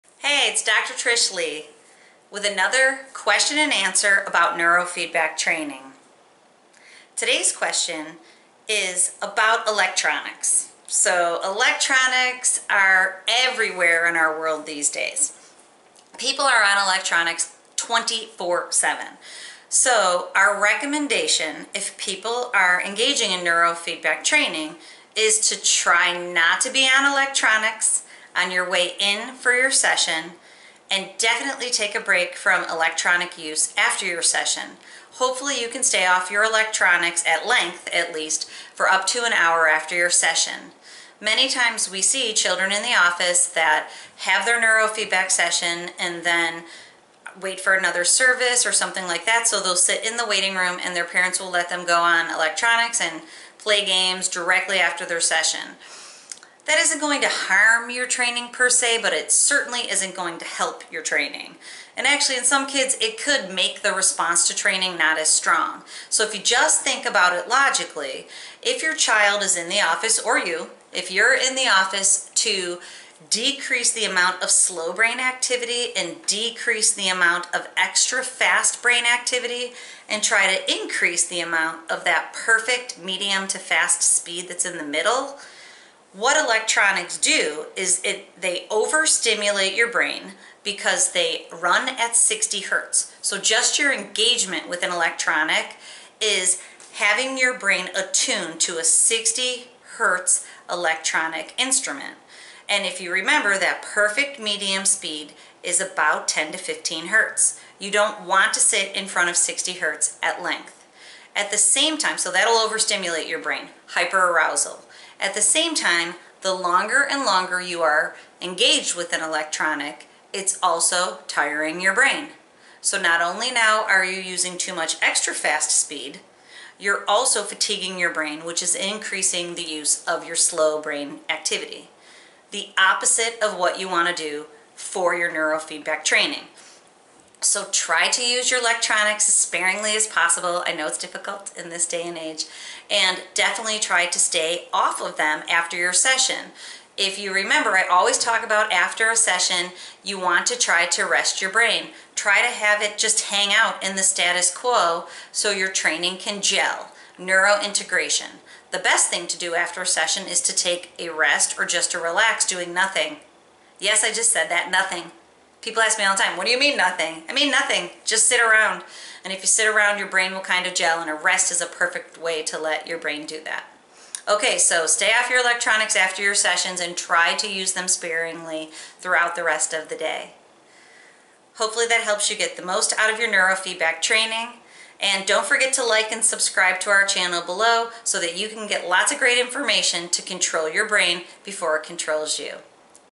What do Electronics do to my brain?
Dr. Trish Leigh discusses elctronic use and how it impacts your brain. This is especially important before and after your Neurofeedback sessions. Check it out now.